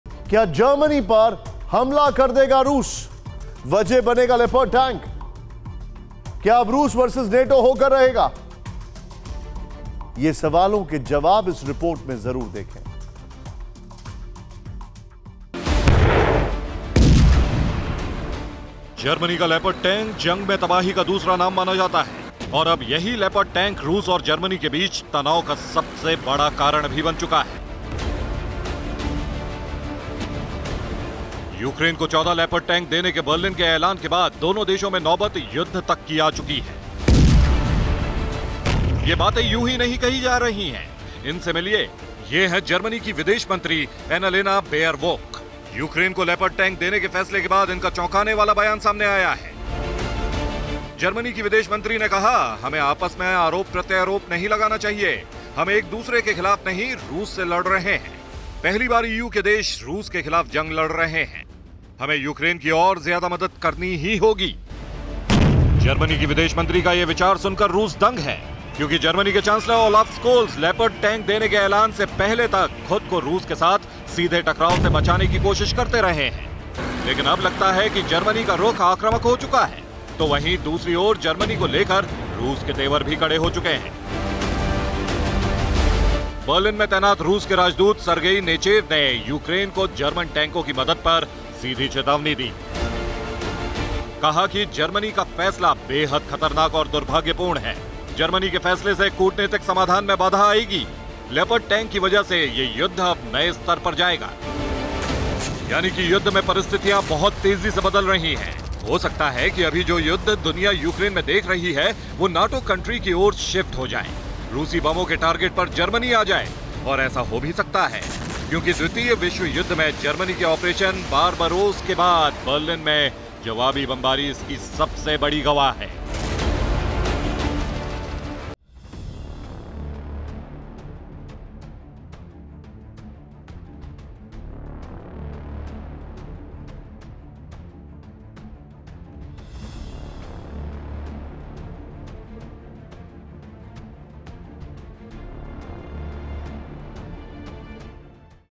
Russia Ukraine War: क्या Germany पर हमला कर देगा Russia ? | Putin | Biden
Russia और Germany में बढ़ते तनाव के बाद अब दो बेहद जरूरी सवाल।. क्या जर्मनी पर हमला कर देगा रूस ?. क्या अब रूस Vs नाटो होकर रहेगा ?. ये रिपोर्ट देखिए